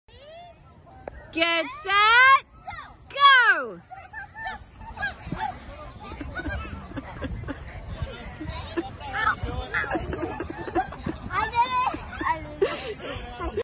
Harvest Festival 2023
Our harvest festival, three legged sack, race, caramel apples, popped, popcorn, doughnut eating contest, local vendors, live music great day to bring the family out to come see us.